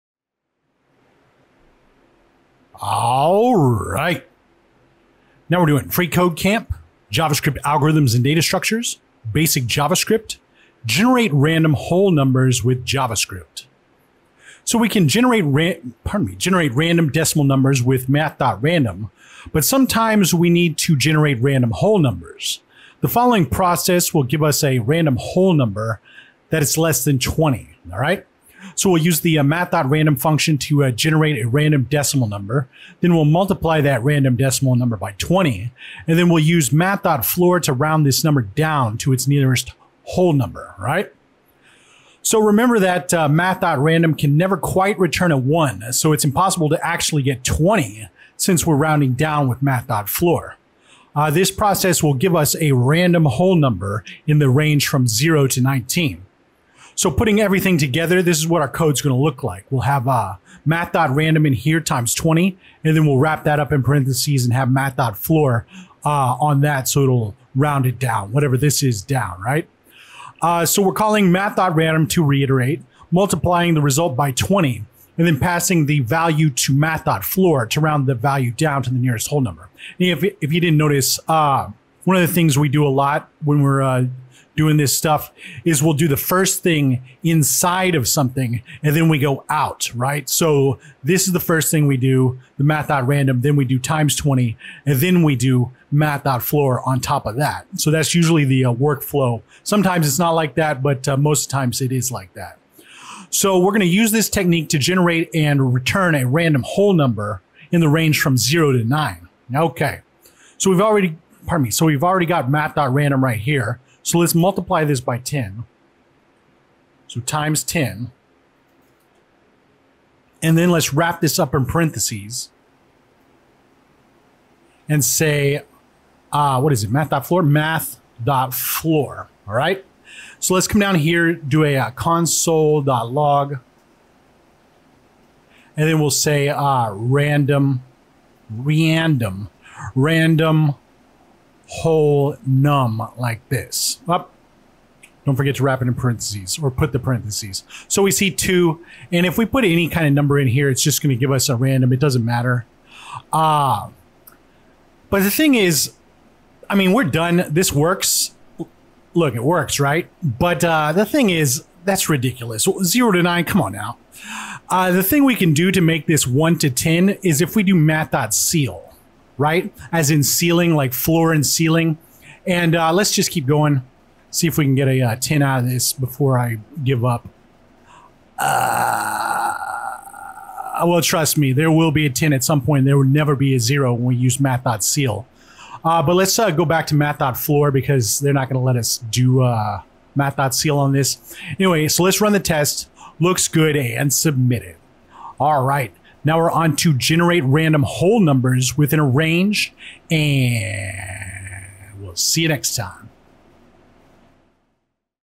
JavaScript Basic 106: Generate Random Whole Numbers with JavaScript | FreeCodeCamp
🚀 **"Whole New World: Craft Whole Numbers with JavaScript's Random Function!"** 🌍🎲

Hello, Code Adventurers! 🌌🔍

Ever wondered about the mystery behind those unpredictable dice rolls and game spins? 🎮🎲 Dive with us into the world of JavaScript, where we'll demystify the enchanting process of crafting random whole numbers. It's not just about getting a number; it's about the journey from decimals to whole digits! ✨

🌟 **Highlights of Our Epic Quest**:

- **Decimals' Day Out**: Discover how `Math.random()` kicks things off with a mysterious decimal that's always shy of reaching 1. 🎩
- **Multiplier Machine**: See how multiplying our shy decimal with a number (like 20) transforms its potential. It's like feeding it a powerful potion! 🍷💫
- **The Floor is... Numbers!**: Dive into the wonders of `Math.floor()`, our trusty guide that ensures our random value safely lands on whole grounds. 🛬🎯
- **Rounding Riddles Resolved**: Learn why, with this method, some numbers like 20 remain the ever-elusive dream. 🌌🔍
- **Your Grand Challenge**: Unleash your newfound knowledge and craft code that concocts a random number dancing between 0 and 9. Do you have the golden touch? 🌟

Join us on this mystical voyage, blending warmth, math, and moments of awe. By the end, not only will you wield the power of randomness, but you'll also have a tale of numbers and decimals to mesmerize your fellow code wizards!